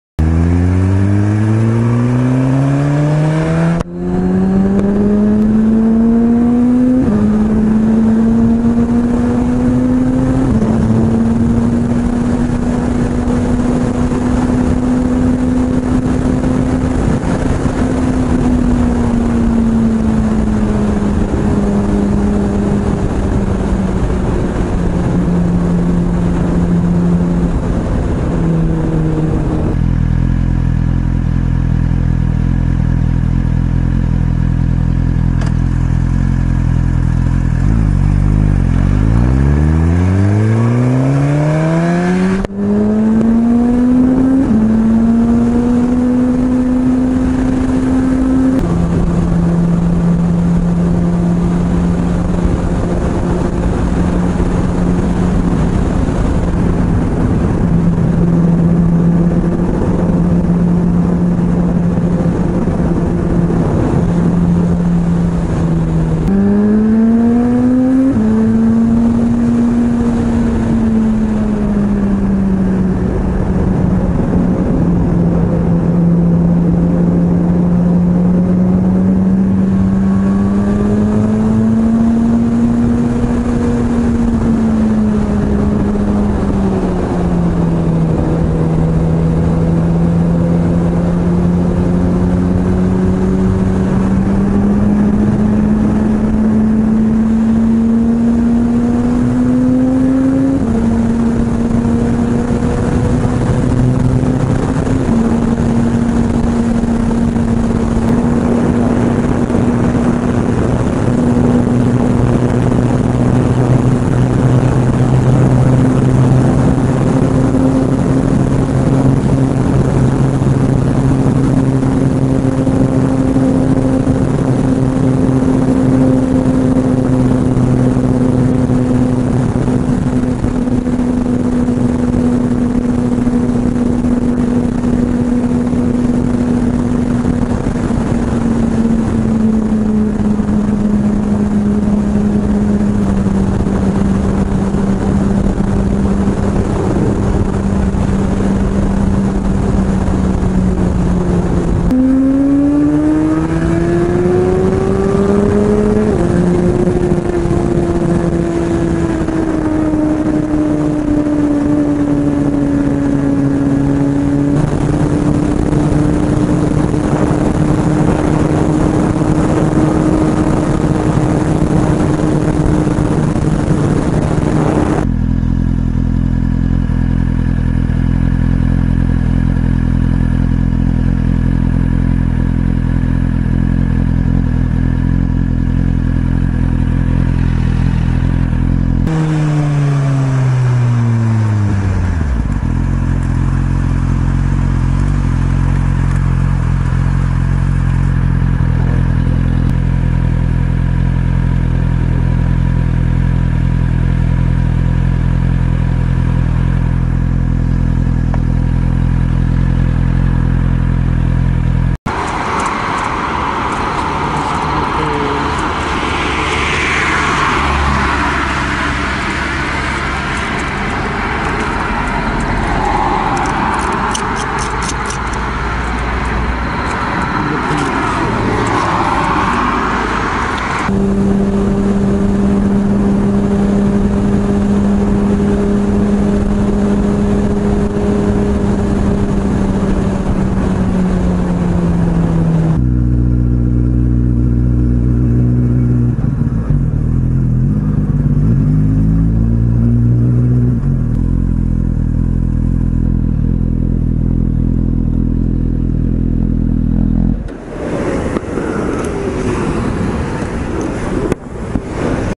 2018 08 05 임랑해수욕장 밤바다 솔투(GSX-R1000R)
여름휴가따윈 없는 회사 귀속 노비는
눈물을 훔치며 밤바다를 보러 떠납니다.
2:58 핸드폰 사망?

SUZUKI; GSX-R1000R L7
SONY; HDR-AS300